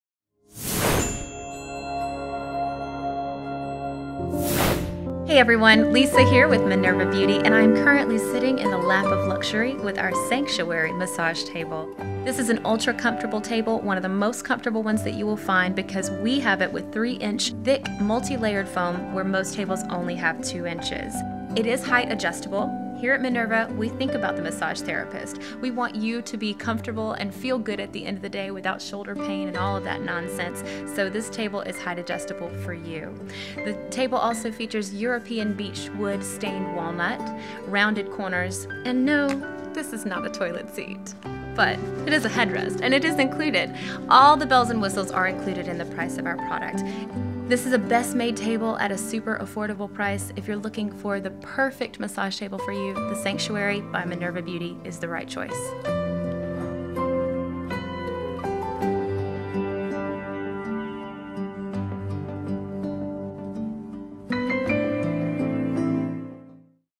Sanctuary Adjustable Deluxe Massage Table | Minerva Beauty
The Sanctuary Adjustable Deluxe Massage Table features height adjustment, closed storage with sliding cabinet doors, and pneumatic lift controlled back and leg rest. Constructed with European Beech wood stained in Walnut, this super comfortable adjustable table has a 3" thick multi-layered cushion and durable black vinyl upholstery. Other features include rounded corners, double leg-lock knobs, and headrest outlets on both ends.